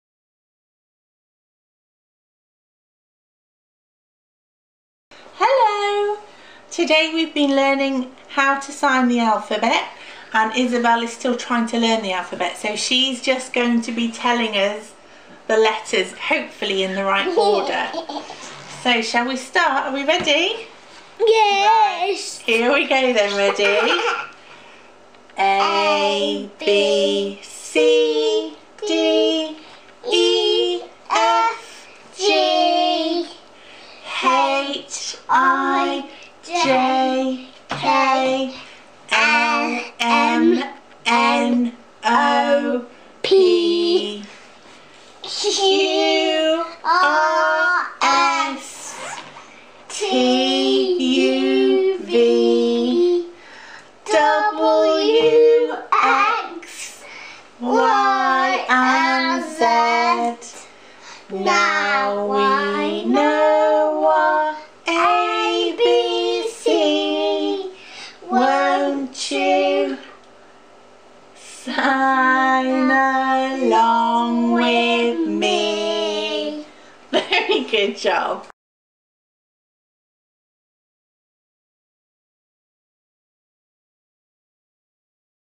Alphabet
Me and Isabelle do our best to remember the alphabet in the correct order!